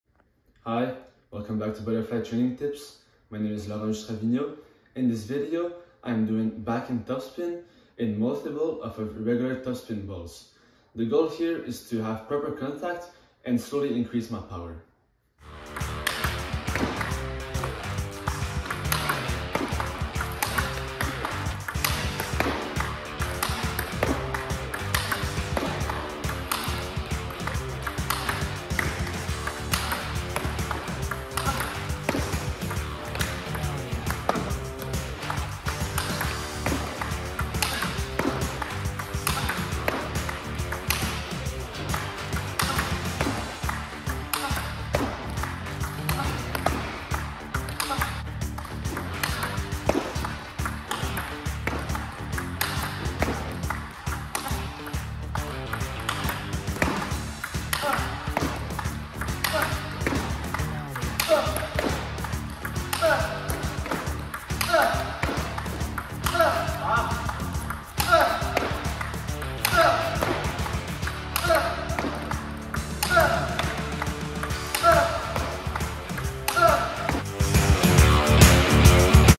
Butterfly Training Tips with Laurent Jutras Vigneault - Power Backhand Loops from topspin
In this Butterfly Training Tips, Laurent Jutras Vigneault is executing Power Backhand Loop from topspin using multi-ball